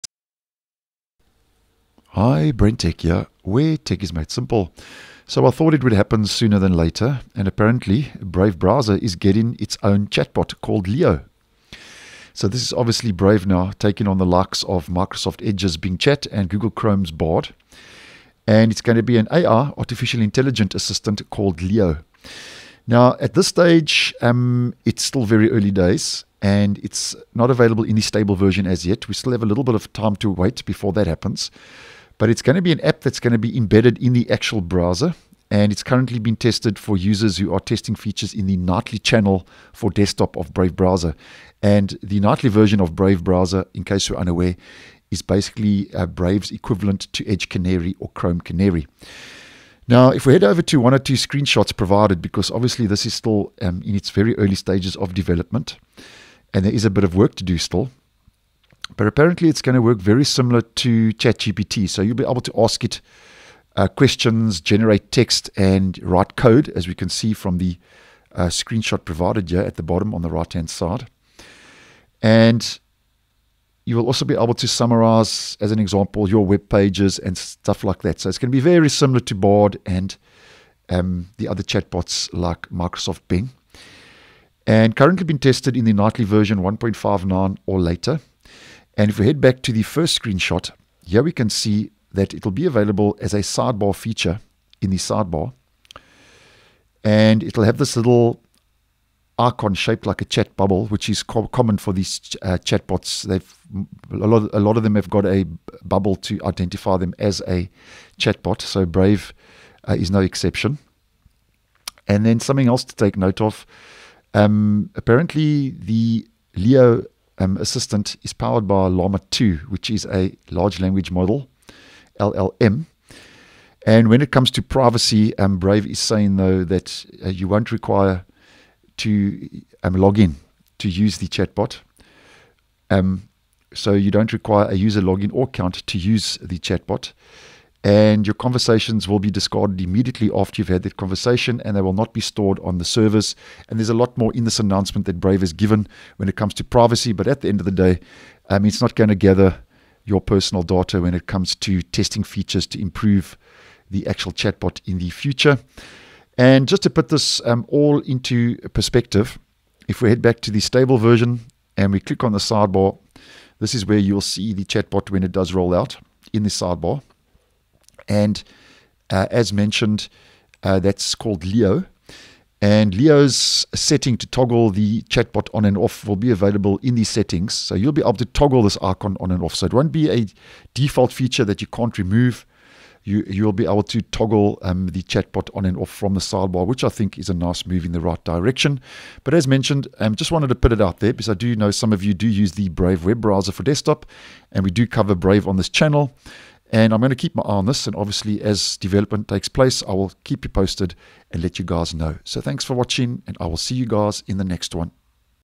Brave Browser is getting it's own native AI Assistant called Leo
As posted by ghacks.net Brave has announced that Leo, the AI assistant built natively in the Brave browser, is now available for testing in the Nightly desktop channel (starting with version 1.59).